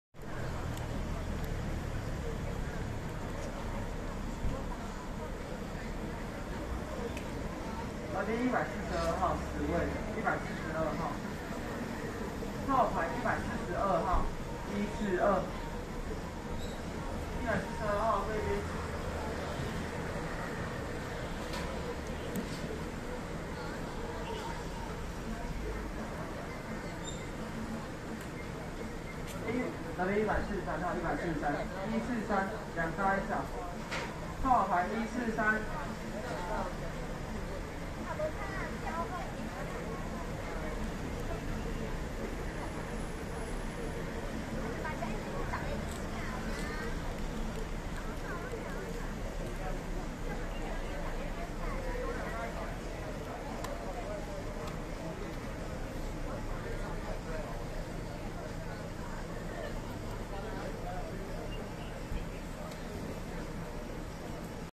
竹香園甕缸雞白河仙草旗艦店
2022年4月4日參觀完台灣國際蘭展後順道來這家餐廳吃晚餐,沒想到餐廳客滿,必須抽號碼牌排隊,服務員告訴我們要等一個鐘頭,只好到隔壁的餐廳吃,它真的是超人氣。